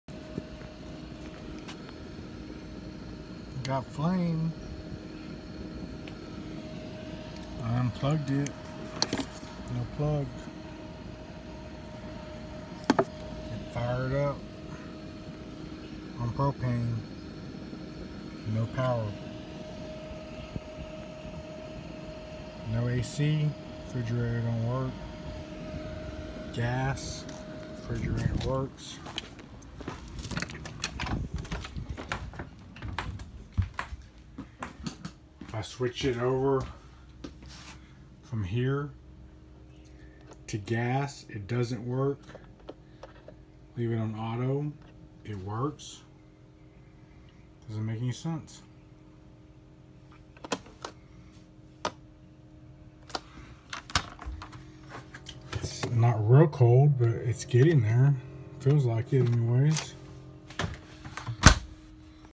2016 Norcold N811FRT not getting cold.
Trouble shooting 2016 Norcold N811FRT Jayco Jayflight 33RBTS.